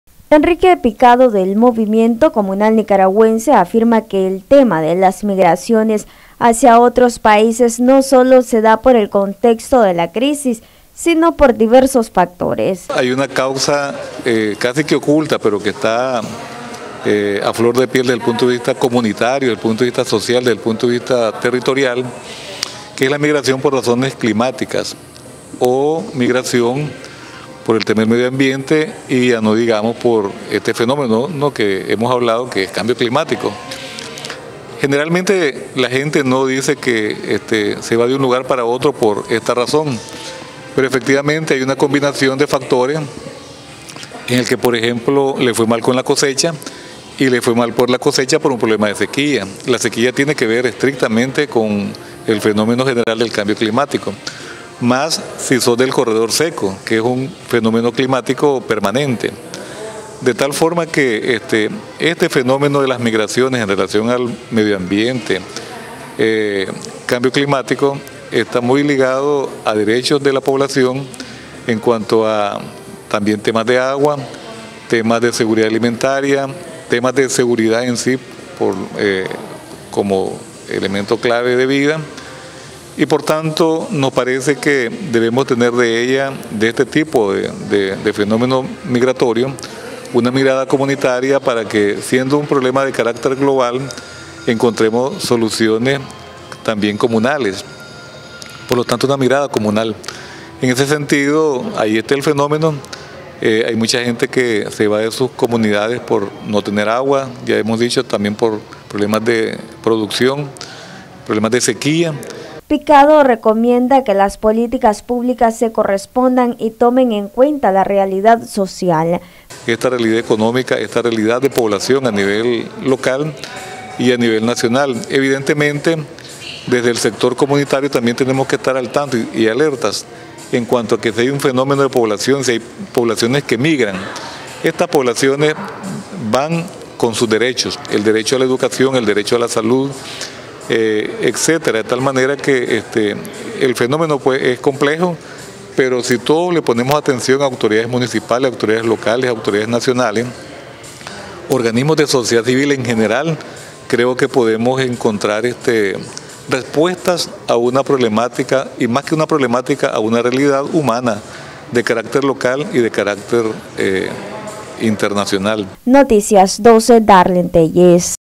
Migraciones obedecen a diferentes factores
Enrique Piado del Movimiento Comunal Nicaragüense, afirma que las migraciones  no se da solamente por el contexto de la crisis sociopolítica  si no por diferentes factores.